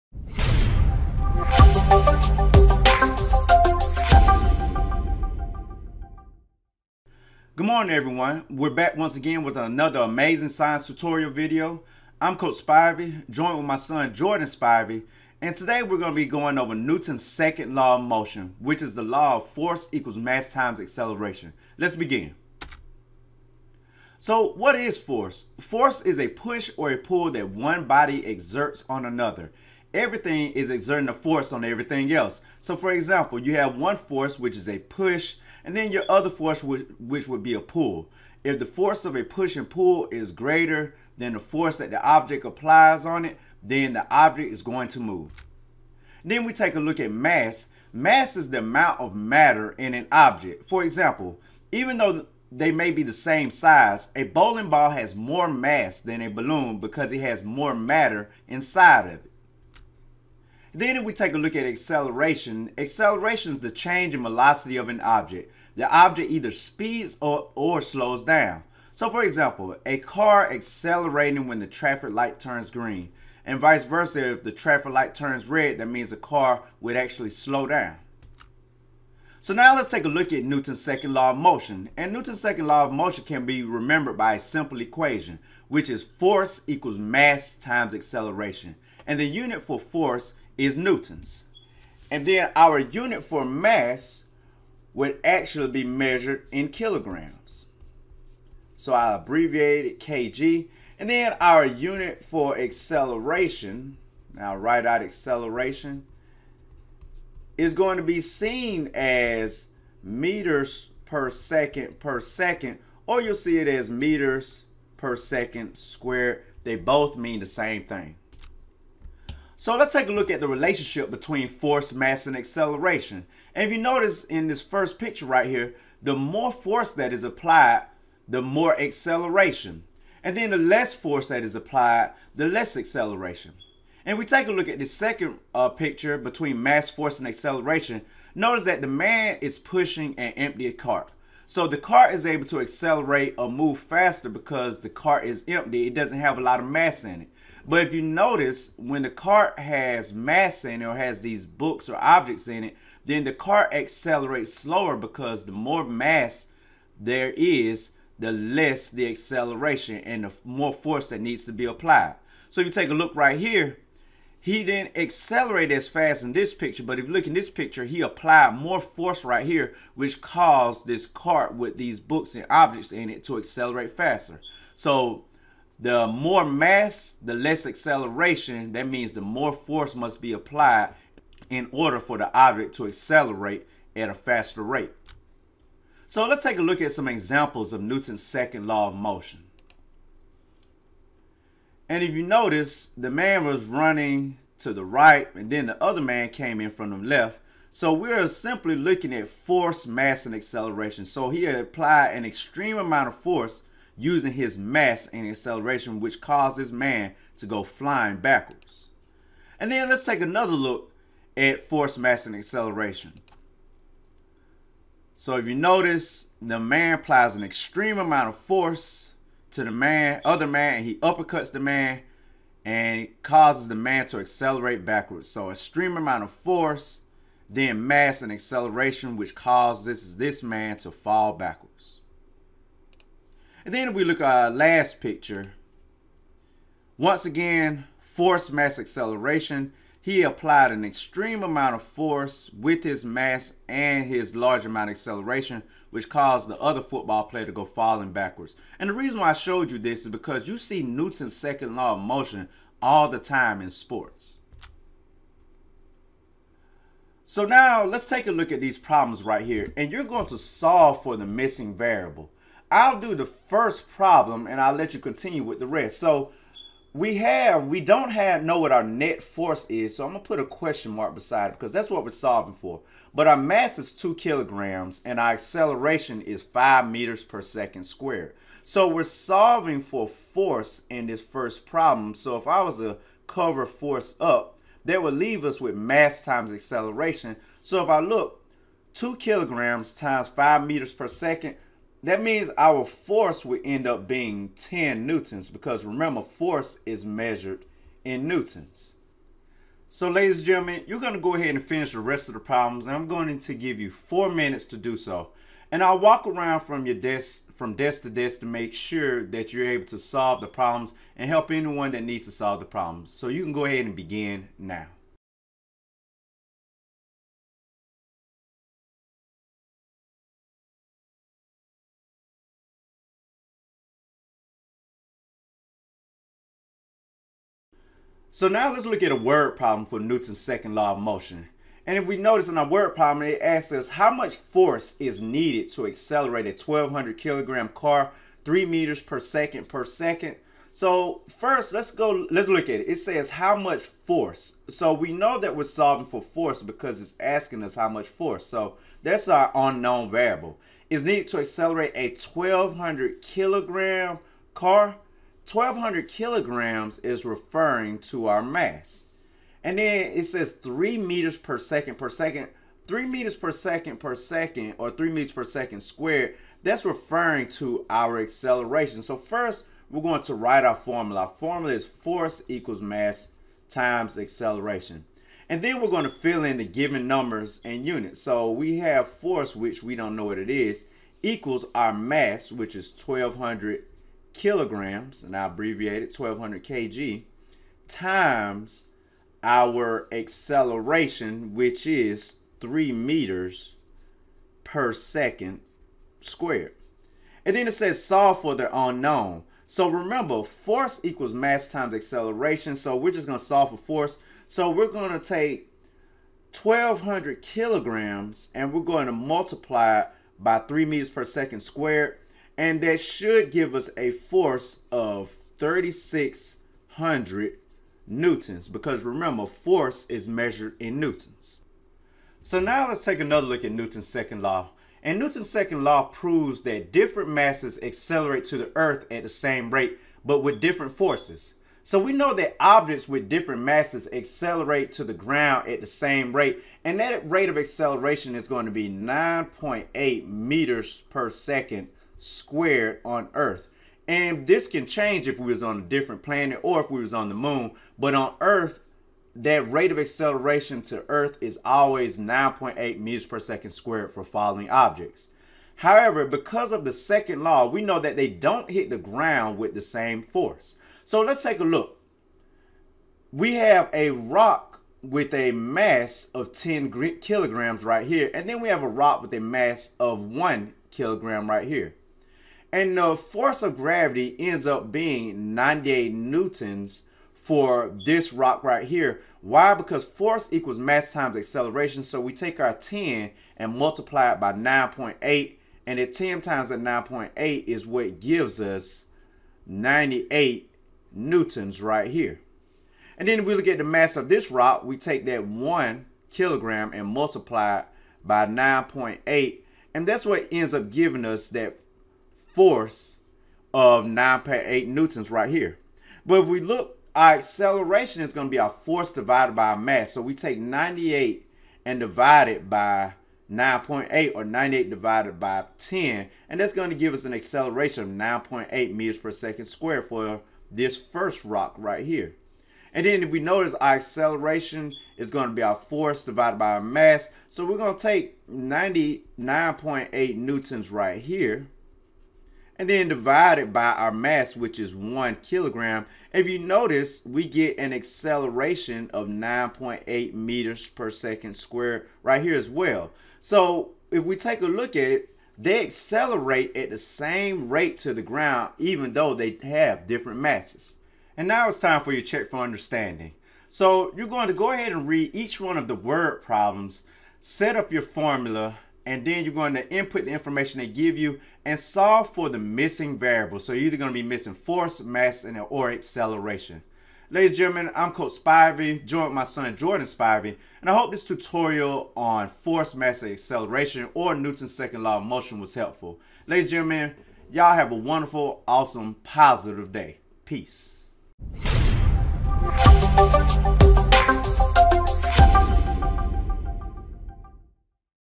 Newton's 2nd Law of Motion 101
Tutorial on Newton's 2nd Law of Motion.  Includes several pictures and animations to help the students understand the relationship between force, mass, and acceleration.  Thanks!!!